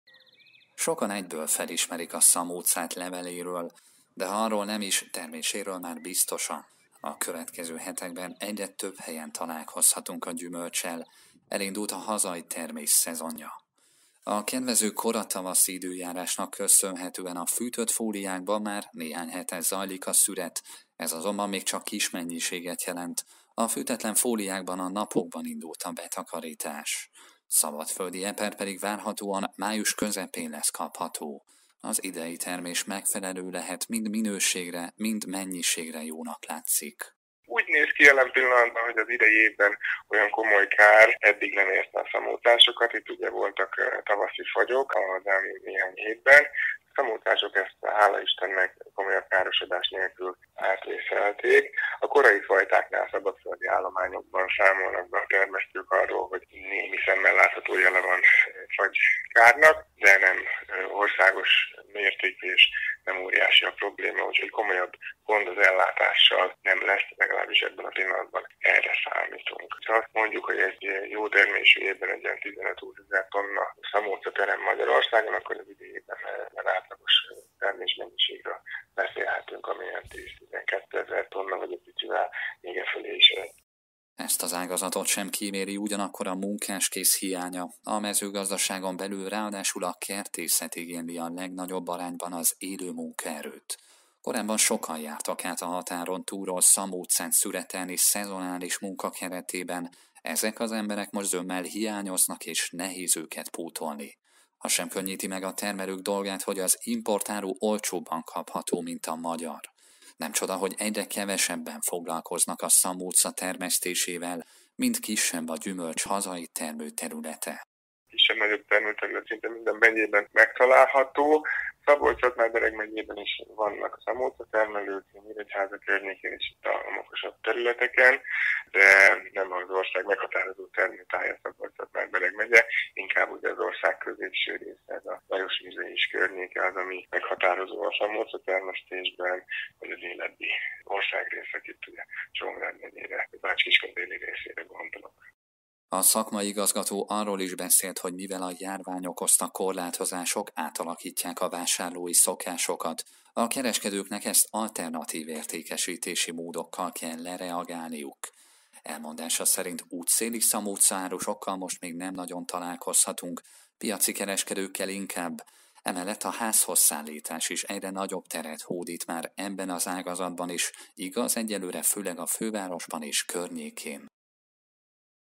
Jónak is, elégnek is látszik az idei magyar szamóca - Kölcsey Televízió
Komoly kár idén eddig nem érte a hazai szamócásokat, a termelők így optimistán várhatják a szezont. A termés minősége és mennyisége is jól alakul. A kertészetet is érintő munkaerőhiány ugyanakkor nem könnyíti meg az ágazatban dolgozók feladatát a szüret idején.